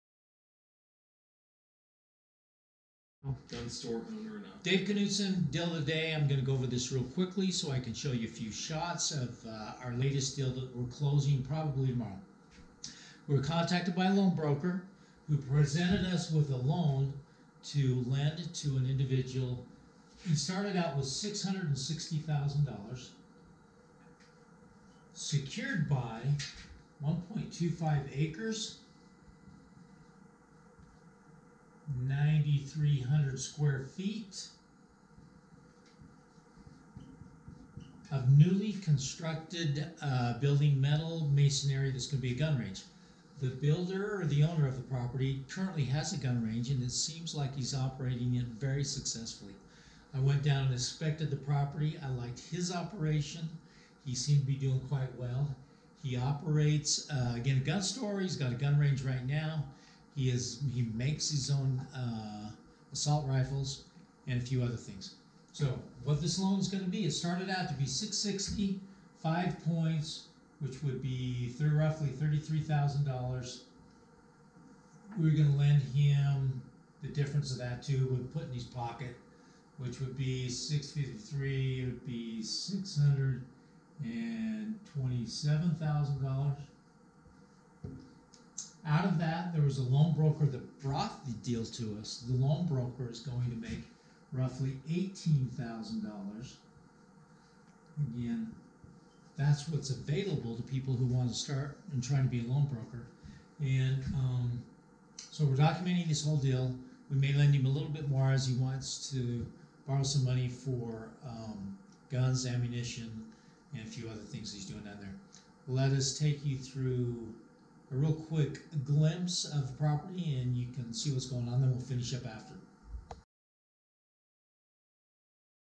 Deal of the Day: Gunrange | Loan Broker Training | Gravity Capital LLC
Deal of the Day! Another description from the Loan Ranger about a deal in Pearland, TX for a new construction indoor shooting range.
RESOURCES & LINKS:
Request a Copy of Dave's book, The Art of the Loan
Dave Knudson deal of the day, I’m going to go over this very quickly so I can show you a few shots of our latest deal that we are closing probably tomorrow.

We were contacted by a loan broker, who presented us with a loan to lend to an individual. We started out with $660,000 secured by 1.25 acres 9,300 square feet of newly constructed building metal masonry. The owner of the property currently has a gun range and looks like he’s operating it very successfully. I went down and inspected the property, I liked his operation. He seemed to be doing quite well, he operates, again, gun stores has a gun range right now, he makes his own rifles and a few other things.

So, what this loan gonna be? So it started out to be 660k, 5 points which will be pretty roughly $33,000. We would lend him the difference of this too which we’ll put in his pocket, which would be  $627,000. Out of that, there’s a loan broker who brought this deal to us, so the loan broker is gonna make roughly $18,000. Again, that’s what’s available to people who want to start and try to be a loan broker, so we’re documenting this whole deal we may lend him a little bit more as he wants to borrow some money for gun ammunition and a few other things he’s doing out there. Let us take you through a real quick glance of a property and you can see what’s going on and and we will finish up after.